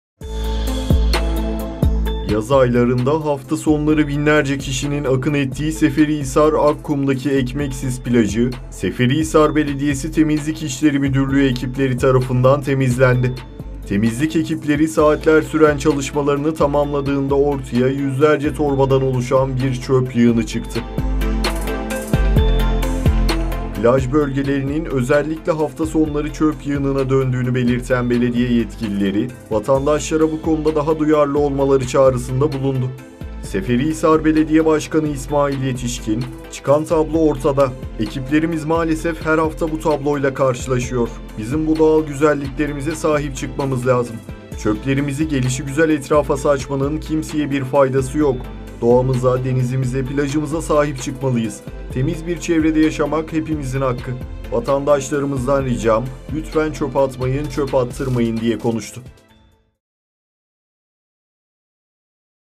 EKMEKSİZ PLAJI ÇÖPE TESLİM!
GÖZDE TATİL KOYLARINDAN SEFERİHİSAR EKMEKSİZ PLAJI; BİRİKEN ÇÖPLERLE GÜNDEMDE. ÖZELLİKLE HAFTA SONLARI ZİYARETÇİ AKININA UĞRAYAN PLAJA SEFERİHİSAR BELEDİYESİ TEMİZLİK İŞLERİ EKİPLERİ GİRDİ… YÜZLERCE TORBA ÇÖP TOPLADI.